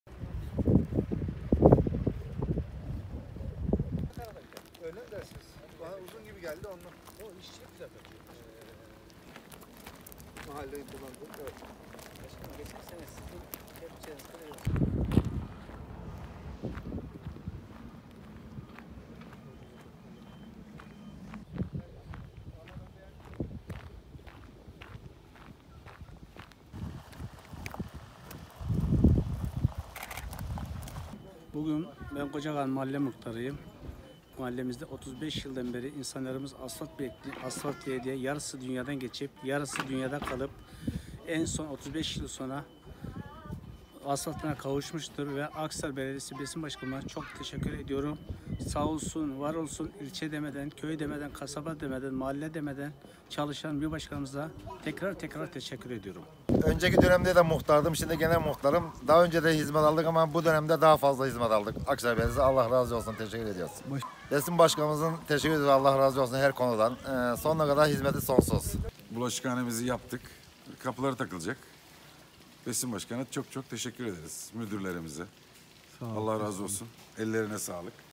Başkan Besim Dutlulu’dan kırsal mahallelere çıkarma
Haber Merkezi

Akhisar Belediye Başkanı Besim Dutlulu, kırsal bölgelerdeki mahalleleri ziyaret ederek çalışmaları yerinde inceledi. Mahalle muhtarları ve vatandaşlar belediye hizmetlerinin önceki yıllara göre çok daha iyi yapıldığını söyledi.

Kocakağan, Sarılar ve Sağrakçı Mahallelerinde yapılan sathi kaplama yol yapım çalışmaları ile yeni yapılan hayır yerlerini yerinde inceleyen Belediye Başkanı Besim Dutlulu’ya Başkan Yardımcısı Bedri Türk ve Kırsal Hizmetler Müdürü Hasan Umutlu da eşlik etti.

Mahalle muhtarları ve mahalle sakinleri ile sohbet ederek istek ve taleplerini dinleyen Belediye Başkanı Besim Dutlulu, “Kırsal bölgelerdeki yaşam standartlarını yükseltmek, burada yaşayan vatandaşlarımızın taleplerini karşılamak için var gücümüz ile çalışıyoruz. Akhisar’da gerek şehir merkezi gerekse kırsal bölgelerde ciddi yol çalışmaları yaptık. Aldığımız talepler doğrultusunda halkımıza hizmetin en kaliteli ve en hızlı şekilde ulaşması için çalışmaktayız ve çalışmaya devam edeceğiz. Halkımızın memnuniyeti ve mutluluğu bize yetiyor. Ekiplerimiz şu anda 35 yıl boyunca yapılamayan bir yolda çalışmalarını sürdürüyor. Kısa süre içerisinde buradaki çalışmamız sona erecek. 35 yıl boyunca yapılmadı ama var güzümüzle çalışarak 1 buçuk yılda yaptık. Bölge halkımızın memnuniyeti bizleri sevindiriyor. Gurur ve mutluluk içerisindeyiz. Emeği geçen tüm arkadaşlarıma teşekkür ederim. Güzel hizmetler yapıyoruz ve yapmaya devam edeceğiz” diye konuştu.

35 YILLIK ASFALT HASRETİ SONA ERDİ

Yapılan yol çalışması ile 35 yıllık hasreti sona erdiren Belediye Başkanı Besim Dutlulu’ya teşekkür eden Kocakağan Mahalle Muhtarı Ahmet Orcan, “35 yıl boyunca asfalt bekleyen vatandaşlarımızın bir kısmı asfalt göremeden vefat ettiler. Kocakağan Mahallemiz ile Pınarcık mezrası arasında bulunan 35 yıldır çile çektiğimiz yol, Belediye Başkanımız Besim Dutlulu’nun talimatları ile hemen yerine getirildi. Mahallemizin 35 yıllık hasreti sona erdi. Allah Besim Başkanımızdan ve ekibinden razı olsun. Şehir merkezi, kırsal mahalle demeden çalışan bir başkanımız var; tüm mahallemiz adına kendisi ve ekibine tekrar tekrar teşekkür ediyorum” dedi.

GEÇEN DÖNEMDEN ÇOK DAHA FAZLA HİZMET ALIYORUZ

Geçen dönemden daha fazla hizmet aldıklarını dile getiren Sarılar Mahalle Muhtarı Sakıp Acar, “Ben önceki dönemde de muhtardım; şimdi yine muhtarım. Daha önceki dönemde de hizmet aldık ama bu dönem çok daha fazla hizmet alıyoruz. Belediye Başkanımızdan Allah razı olsun; her konuda bizlerin isteklerini dinleyerek yerine getirmesi bizleri çok mutlu ediyor. Mahallemiz adına Besim Başkanımıza çok teşekkür ediyorum” dedi.

Sağrakçı Mahallesi Muhtarı Mesut Atasoy, Akhisar Belediye Başkanı Besim Dutlulu’ya ilettikleri her konunun takipçisi olduğuna vurgu yaparak teşekkürlerini iletti.